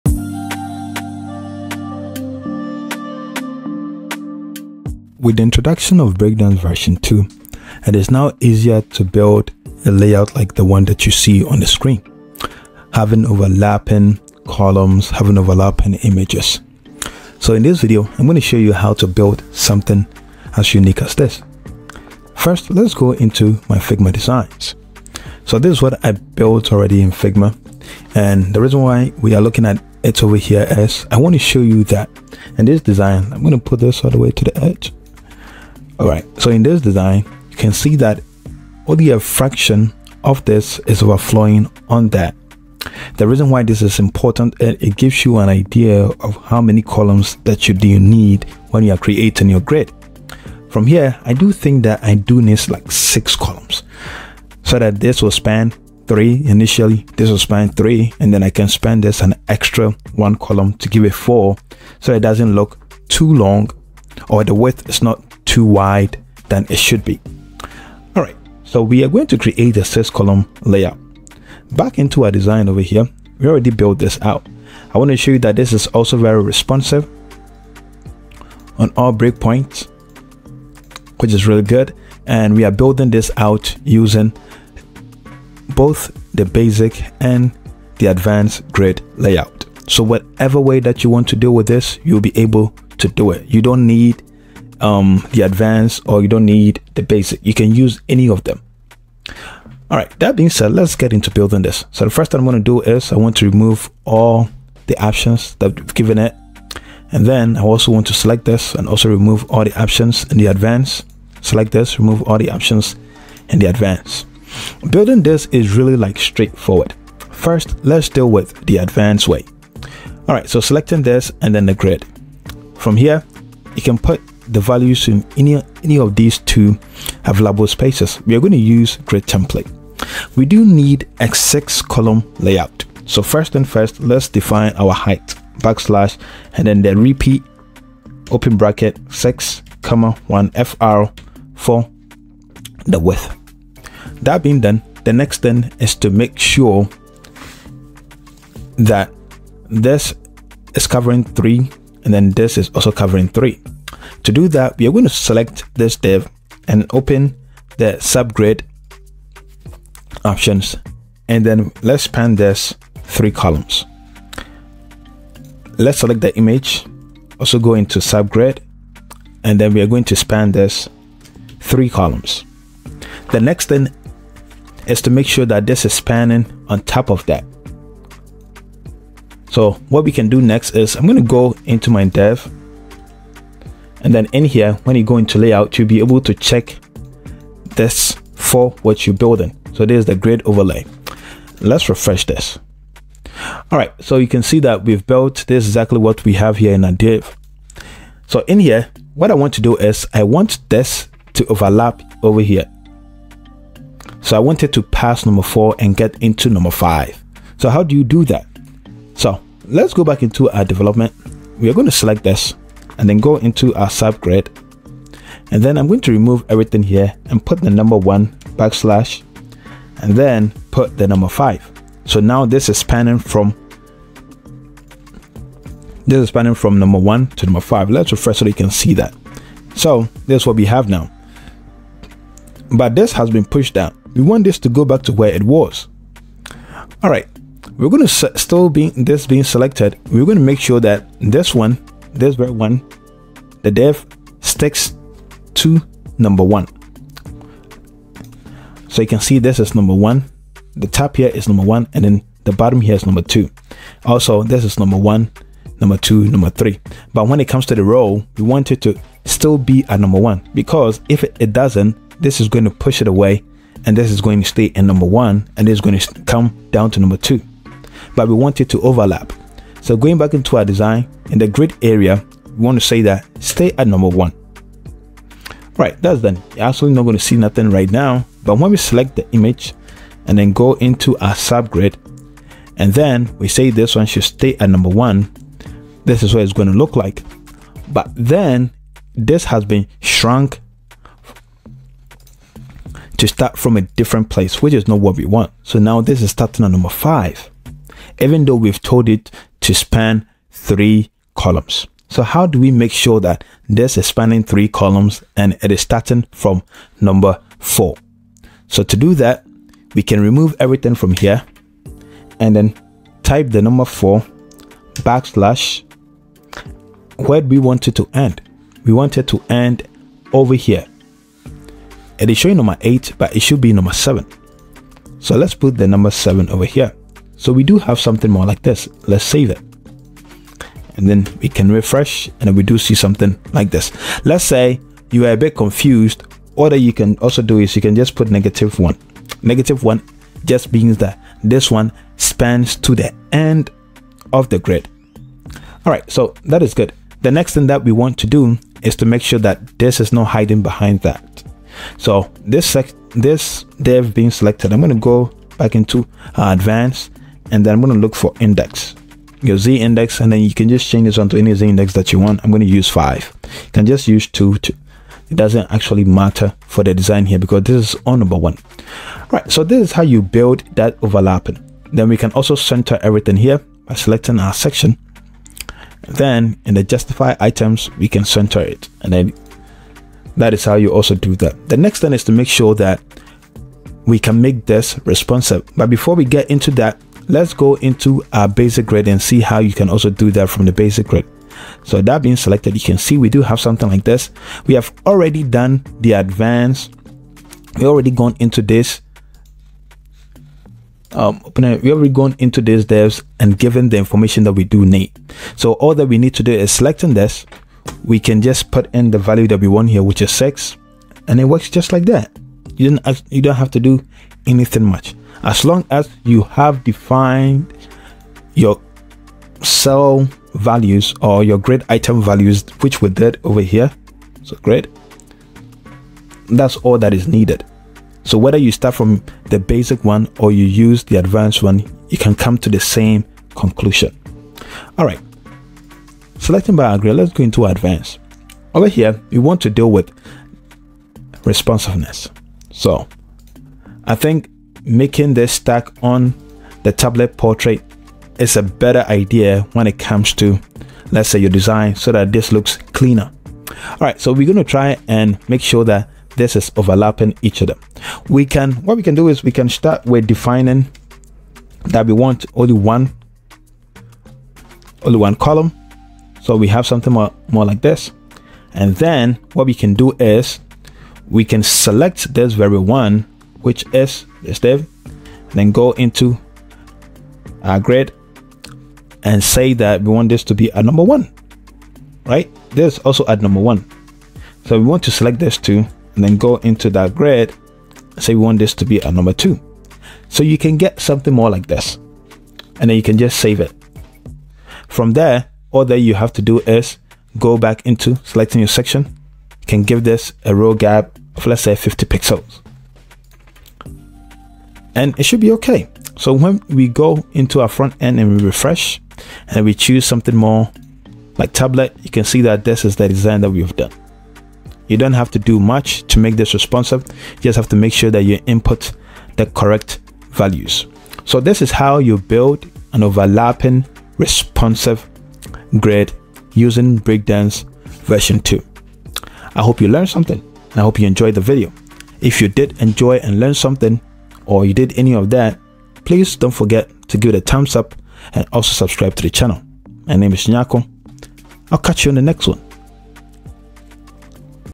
Mastering Overlapping Grids with Breakdance
Dive into the world of overlapping grids with my latest tutorial using the Breakdance's CSS Grid! This video will guide you through the steps of creating with either the basic or advanced grids option.

Video Timeline:
0:00 - Introduction
0:22 - Figma Overview
1:55 - Advanced Grid Options
2: 08 - Laying Out the Grid
2: 23 - Spanning the Grid Cells
3: 00 - Inspecting & Creating the Overlap
6: 27 - Basic Grid Layouts
7: 18 - Responsive Grid Overlap
8:40 - Conculsion

Have questions or want to see more on a specific topic?